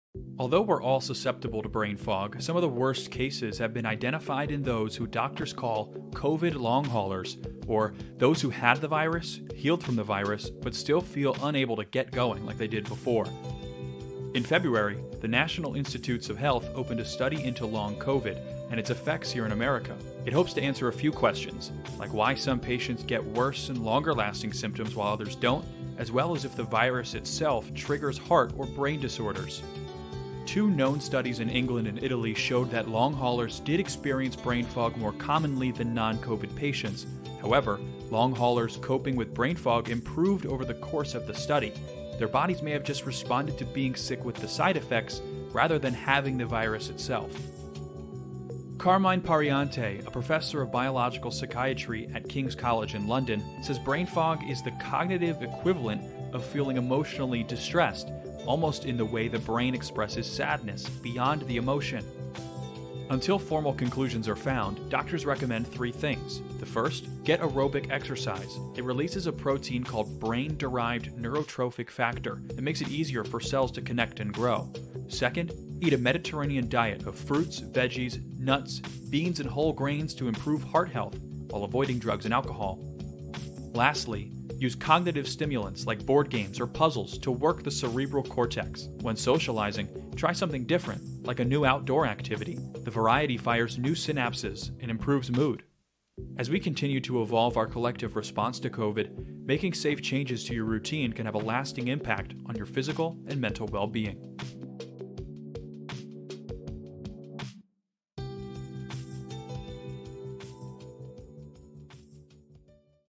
COVID-19 long-haulers and brain fog | Cronkite News
Everyone is susceptible to "brain fog," but researchers in the U.S., Italy and England are looking into the connections with COVID-19 "long-haulers."

(Video by Simon Williams/Cronkite News in collaboration with ABC15 Arizona)